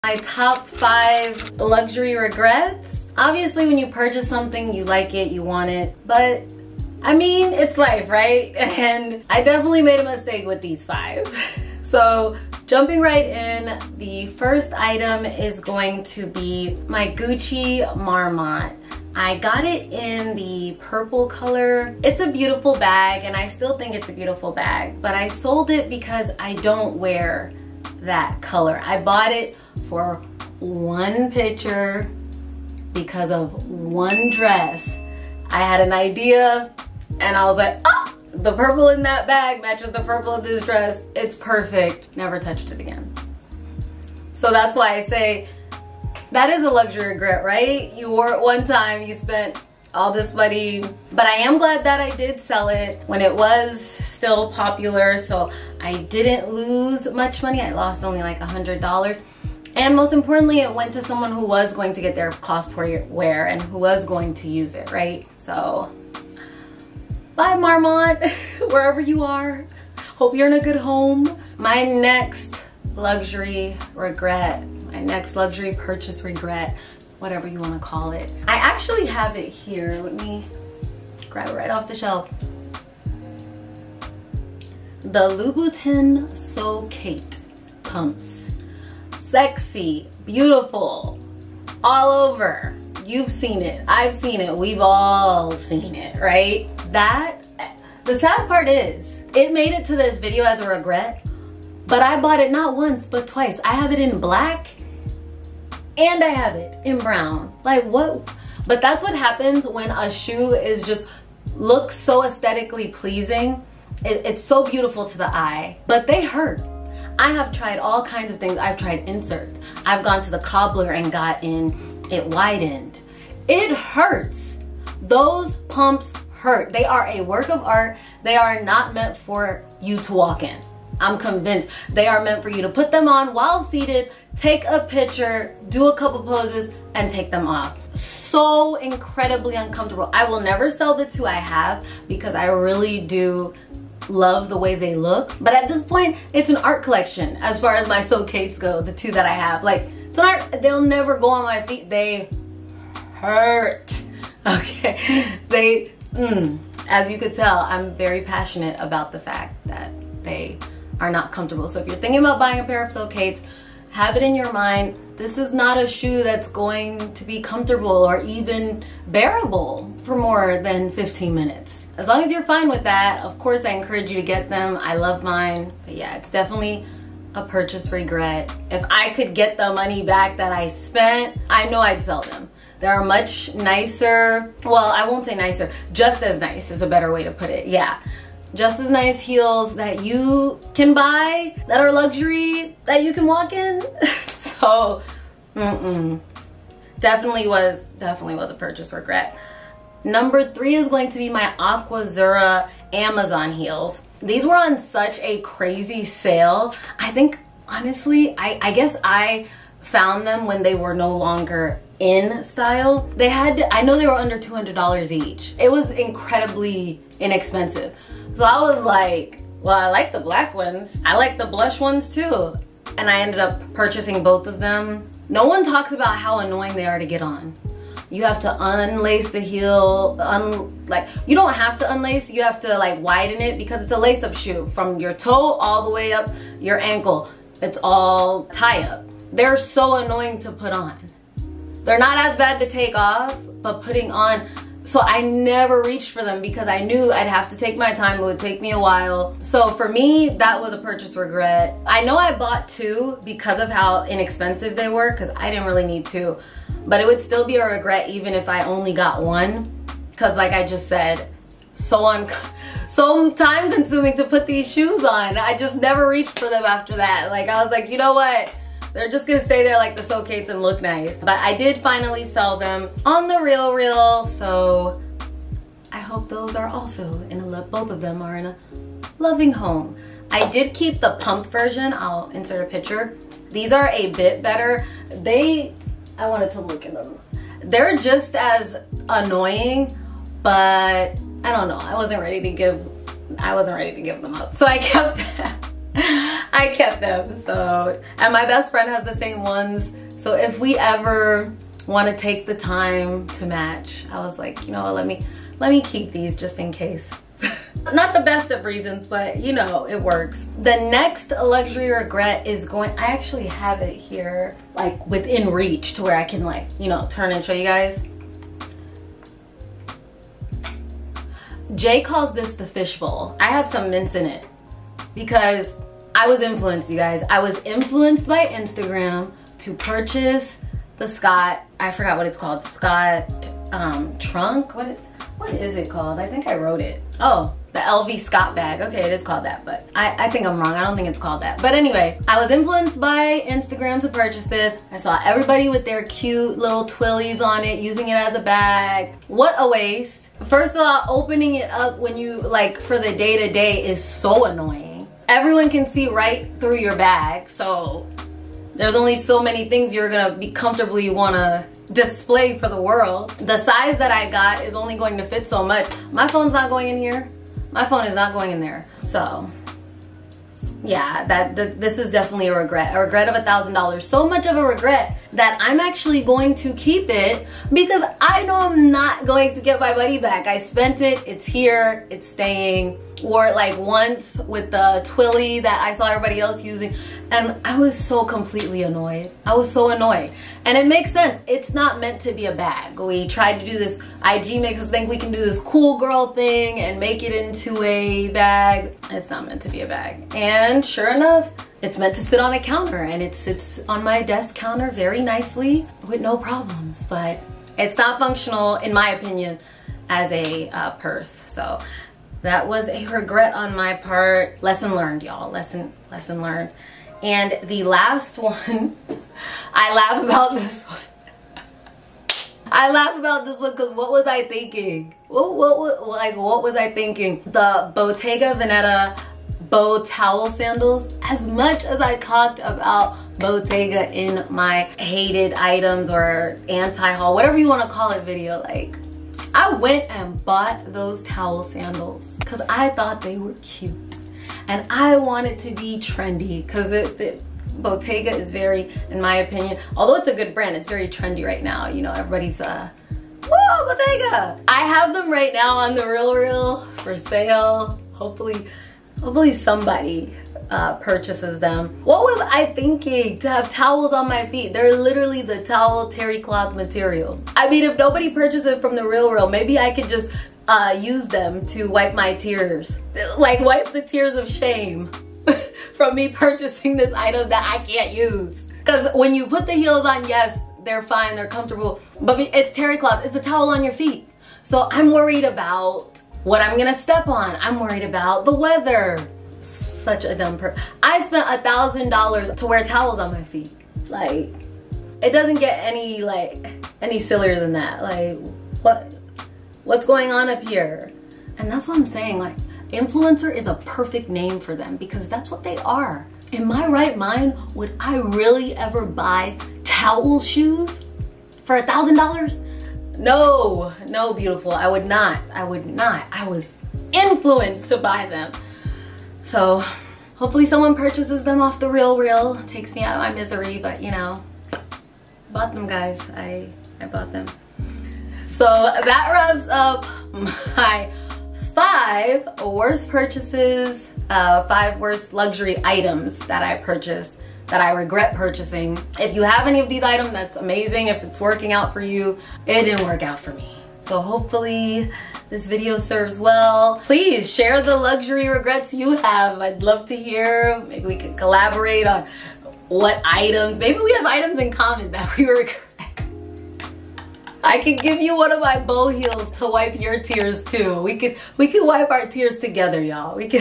Luxury Regrets | I Wish I Didn't Buy These | Louis Vuitton, Bottega Veneta, & More | Arnell Marie
In this video, I'm going to share with you some of the most luxurious things I've ever bought and then regretfully sold/given away. From Louis Vuitton to Bottega Veneta, I'll show you all of the things I wish I'd never spent my money on, and reflect on why I should have done things differently. By sharing my luxury regrets, I hope to give you some perspective on what not to buy and help you make better financial decisions in the future!. Lesson learned!